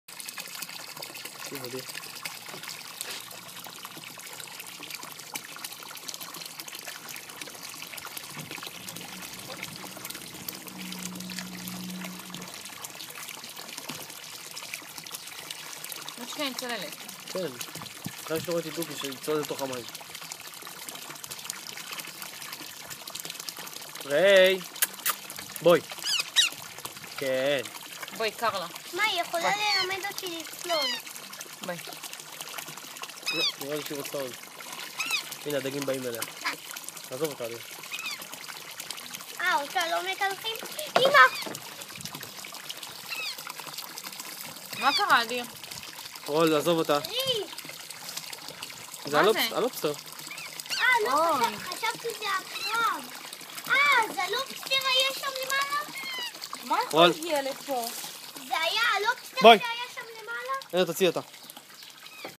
indian ringneck parakeet
My indian ringneck parakeet taking a bath at the fish pond